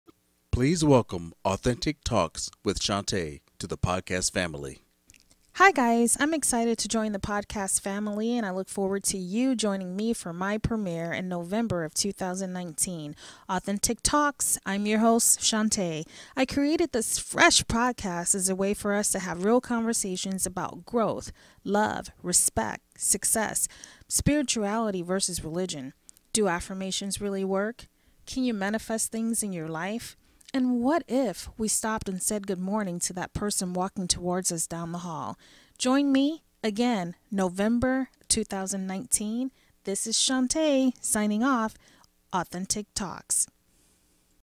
Authentic Talks with Shanta Trailer
Inspiring podcast where we discuss self love, respect, personal growth and adapting to an ever changing society. Authentic talks was created to share steps necessary to get back to fulfilling your life's purpose.